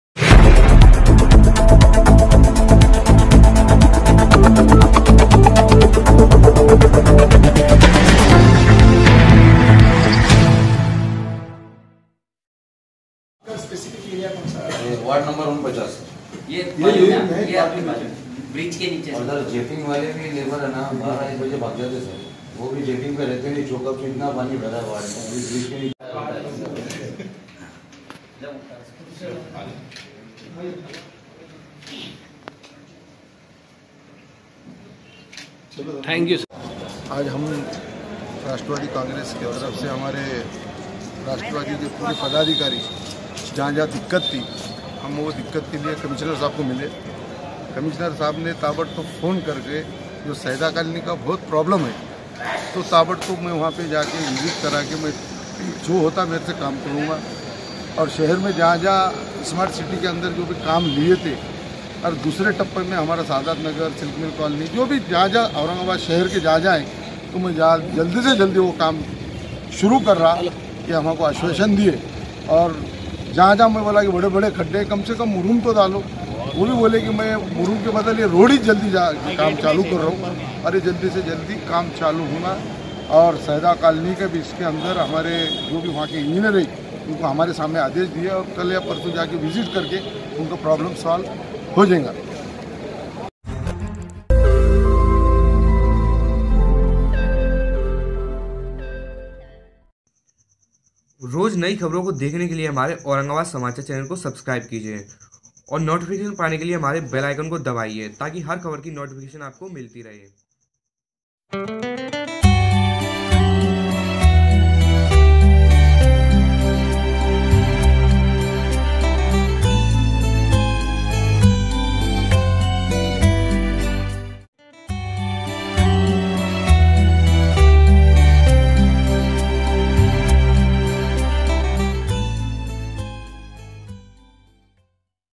शहर के नागरिक मुलभुत सुविधाओं से वंचित NCP की मनपा को चेतावनी साईदा काँलोनी भड़कल गेट की समस्या गंभीर
औरंगाबाद के नागरिक मुलभुत सुविधाओं से वंचित NCP की मनपा को चेतावनी साईदा काँलोनी भड़कल गेट की समस्या गंभीर